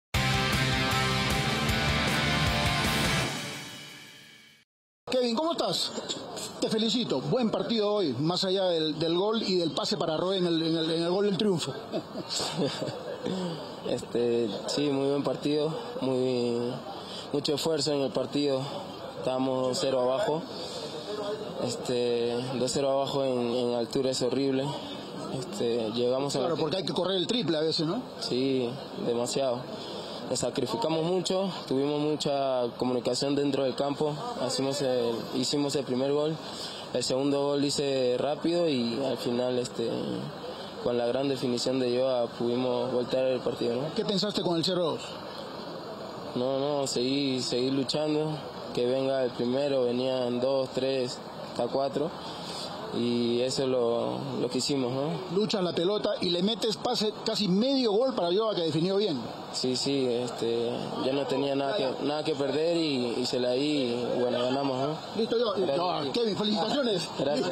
Kevin Quevedo: "Ya no tenía nada que perder, se la di a Joazinho Arroé y ganamos"
Esta noche Gerardo Flores estuvo presente en la llegada de la delegación de Alianza Lima al aeropuerto de Arequipa luego de la victoria 3-2 sobre Melgar. Allí el enviado especial de GOLPERU para este partidazo conversó con Kevin Quevedo que fue autor de uno de los goles y además habilitó a Joazinho Arroé en el tanto que significó la remontada.